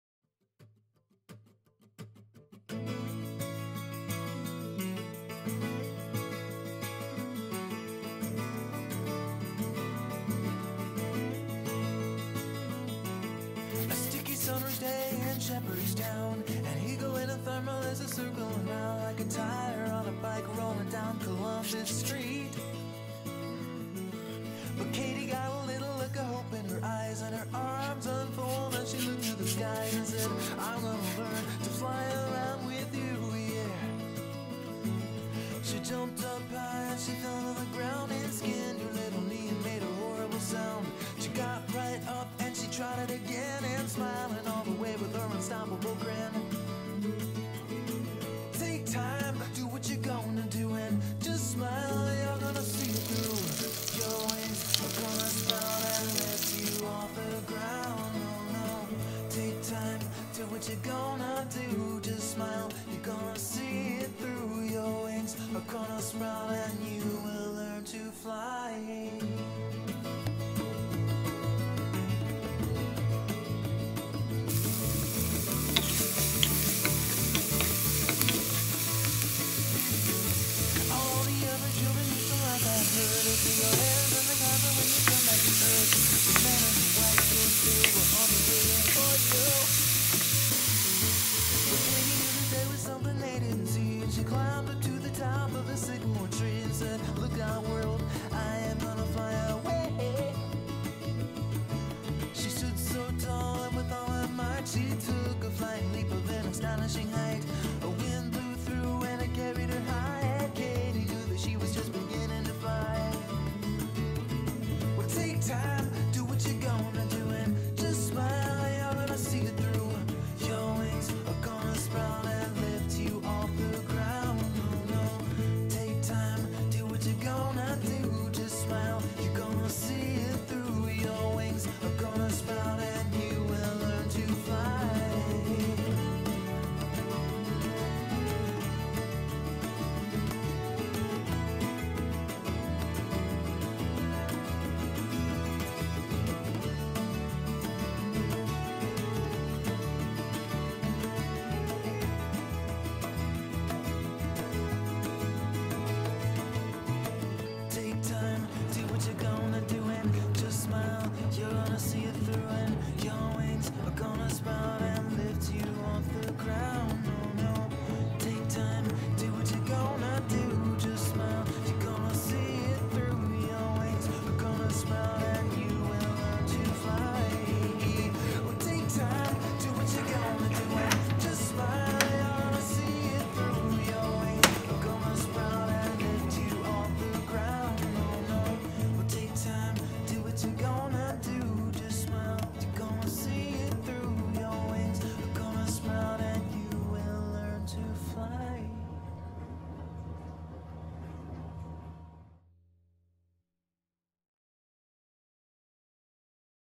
鸡肉青椒韭菜饼
大青椒半个
鸡胸肉大约35克
韭菜4-5根
调料：盐，酱油，酒